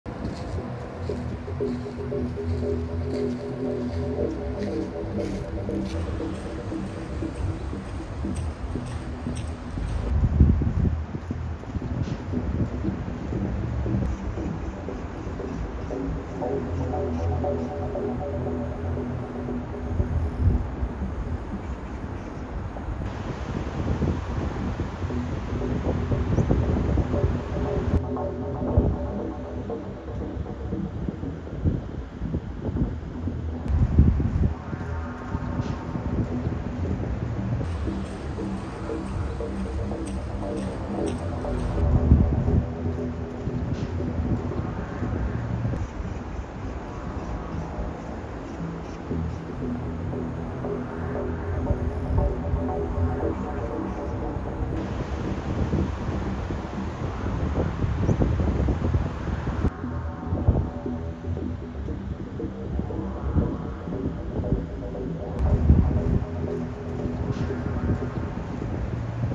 [실시간 현장] 경북 칠곡군 낙동강 호국의 다리 홍수주의보 현장
오늘 오전 10시 20분 낙동강 경북 칠곡군 호국의 다리 지점에 홍수주의보를 발령했다.